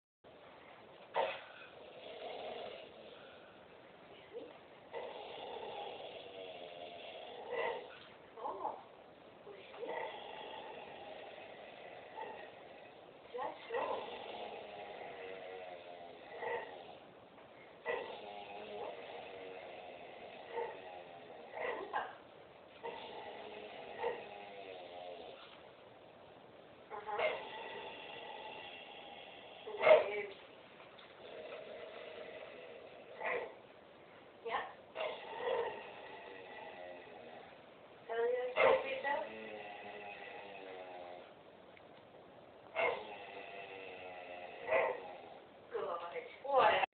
Loki Speaking
Our wonder dog Loki performing his speaking trick on command